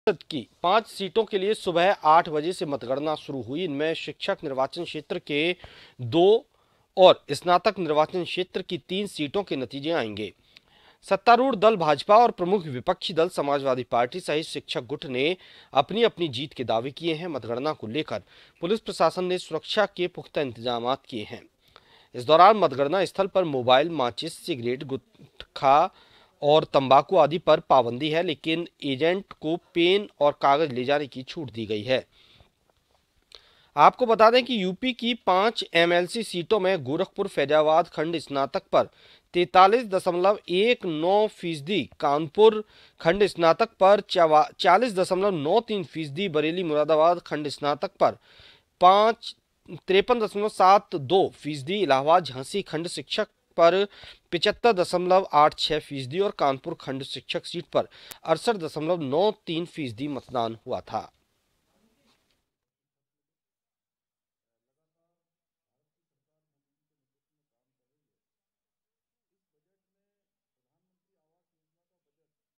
Uttar Pradesh News | उत्तर प्रदेश MLC चुनाव-2023, स्नातक-शिक्षक MLC की पांच सीटों के लिए मतगणना
News Credit-KM